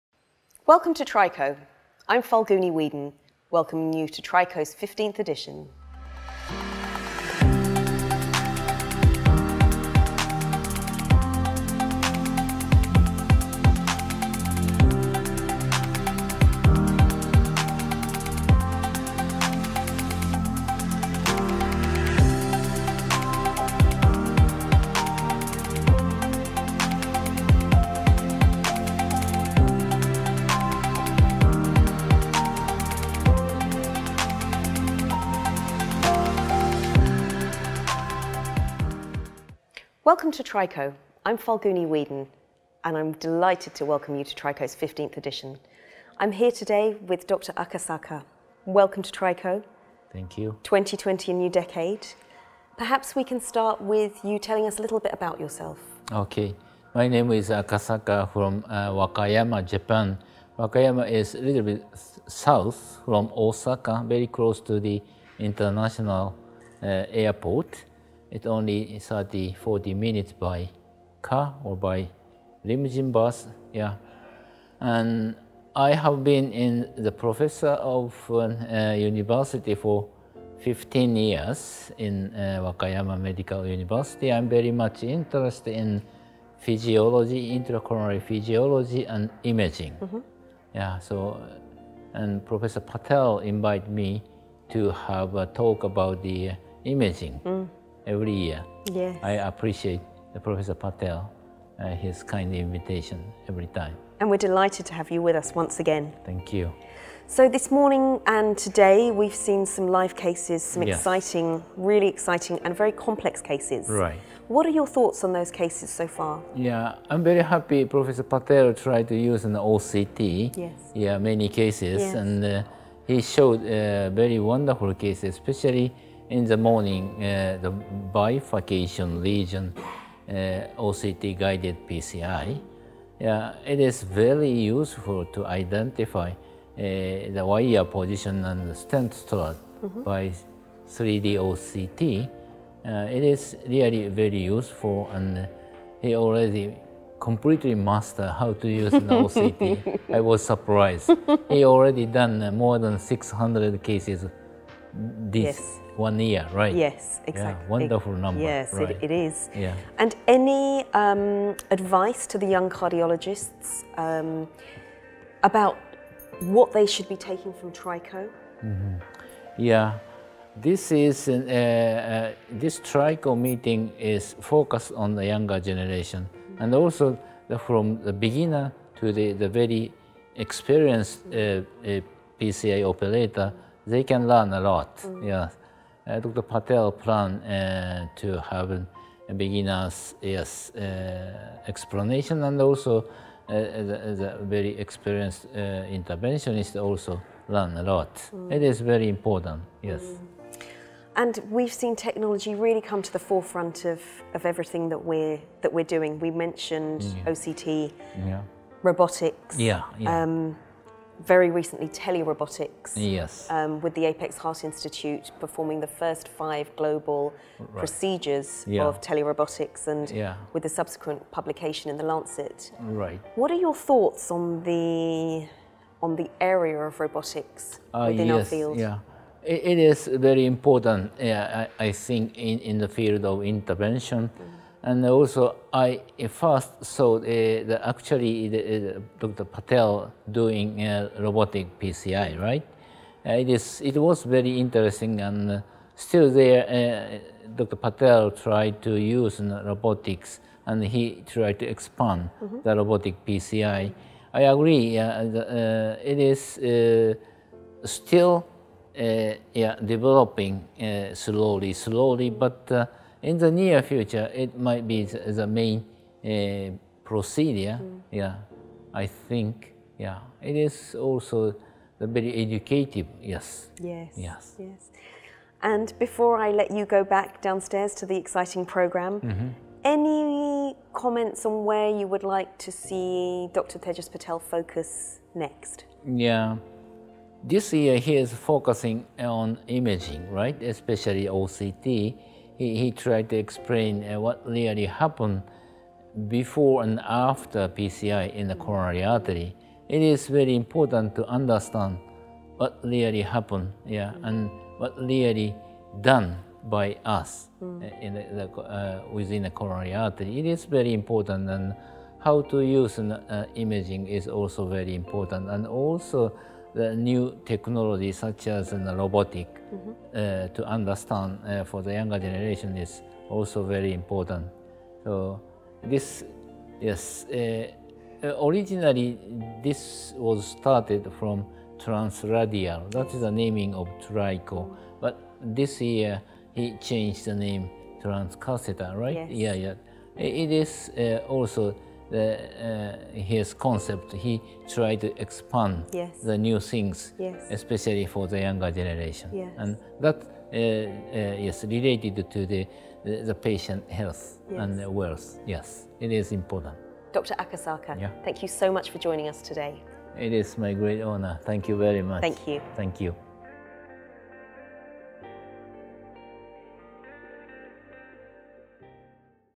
Dr. Takashi Akasaka, MD, PhD. (TRICO 2020)
Professor of Cardiology, Wakayama Medical University
Wakayama, Japan

International Guest Faculty Interview during 15th International Transcatheter Interventional Course (TRICO 2020), live from Apex Heart Institute, Ahmedabad, India.
Interviews are conducted by Mrs. Falguni Wheadon, London, UK.